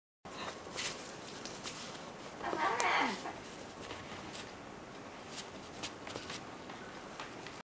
紐が絡んだタイハクオウム
おせちの飾りの紐です。
遊んでいたら・・・アレレ(ﾟДﾟ;)